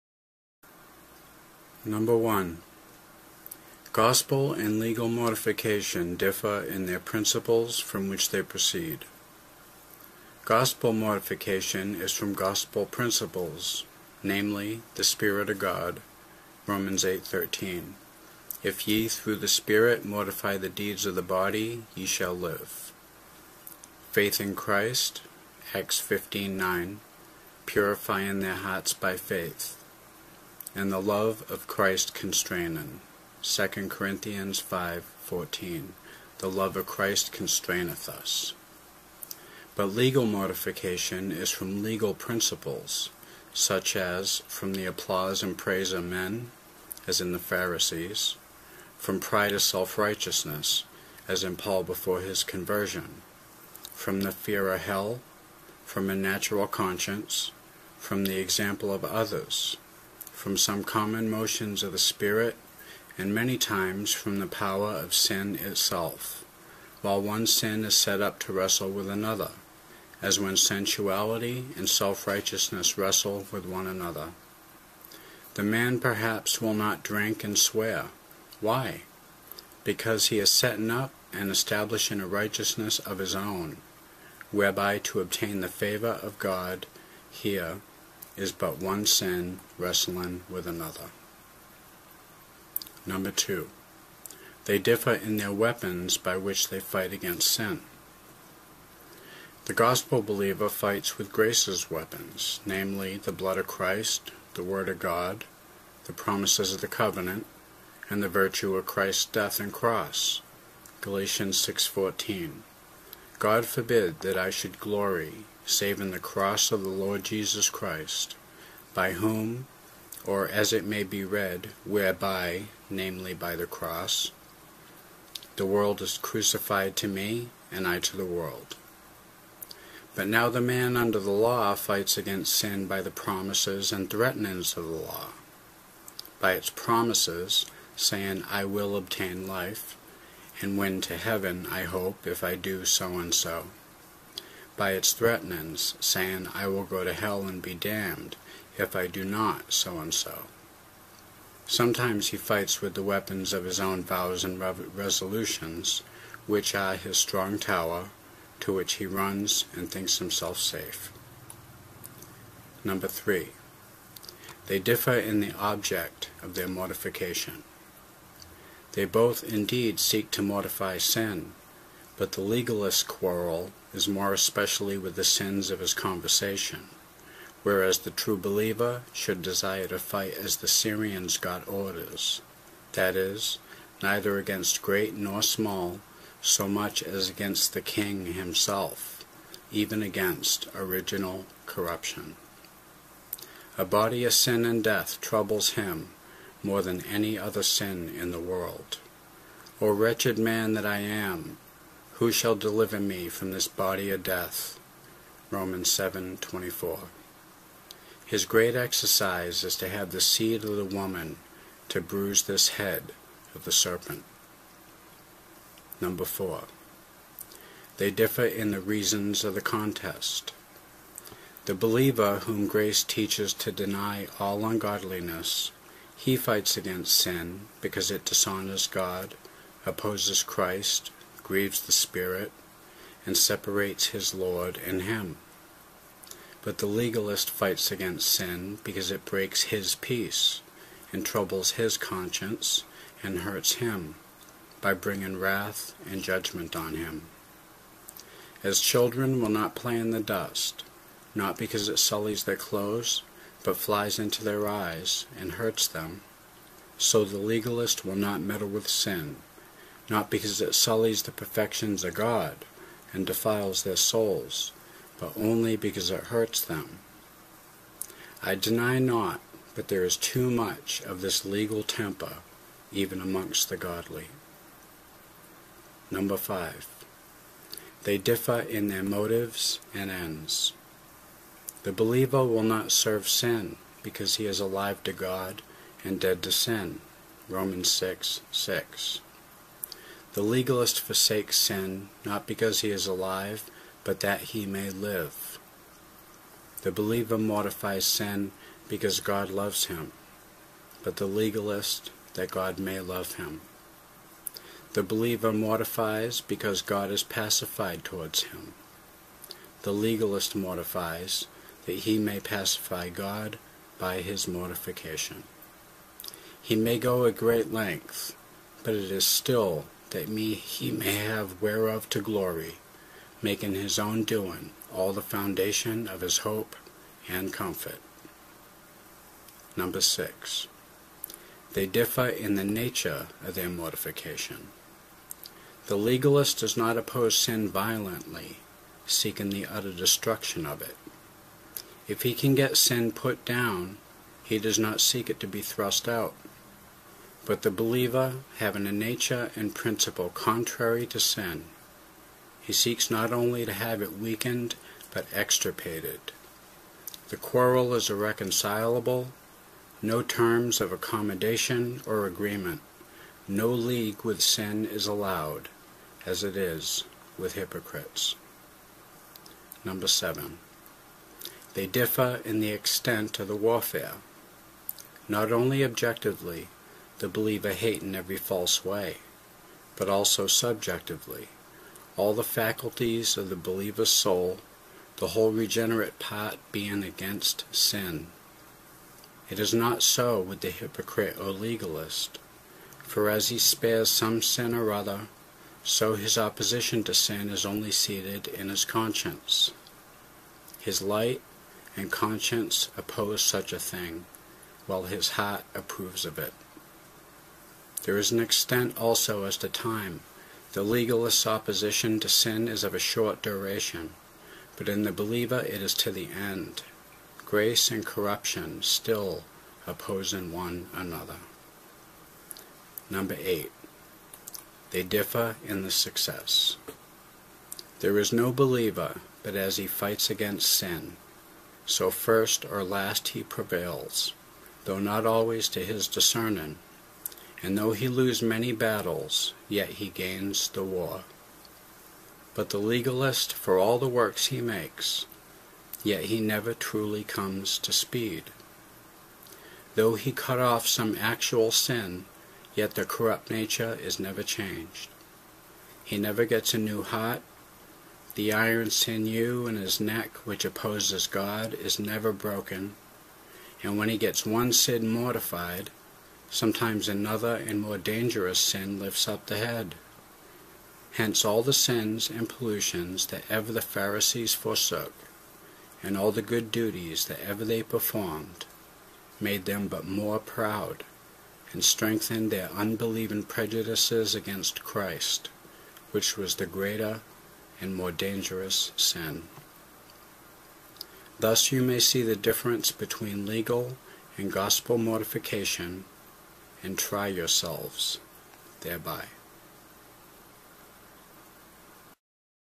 The Difference Between Legal and Gospel Mortification, by Ralph Erskine.
But if ye through the Spirit do mortify the deeds of the body, ye shall live.
~ Romans 8:13 c, d

There is therefore now no condemnation to them which are in Christ Jesus, who walk not after the flesh, but after the Spirit.
~ Romans 8:1

1. Gospel and legal mortification differ in their principles from which they proceed. Gospel mortification is from gospel principles, viz. the Spirit of God [Rom. 8. 13], 'If ye through the Spirit mortify the deeds of the body, ye shall live'; Faith in Christ [Acts 15. 9], 'Purifying their hearts by faith'; The love of Christ constraining [2 Cor. 5. 14], 'The love of Christ constraineth us.' But legal mortification is from legal principles such as, from the applause and praise of men, as in the Pharisees; from pride of self-righteousness, as in Paul before his conversion; from the fear of hell; from a natural conscience; from the example of others; from some common motions of the Spirit; and many times from the power of sin itself, while one sin is set up to wrestle with another, as when sensuality and self-righteousness wrestle with one another. The man, perhaps, will not drink and swear. Why? Because he is setting up and establishing a righteousness of his own, whereby to obtain the favour of God here is but one sin wrestling with another.

takeupcross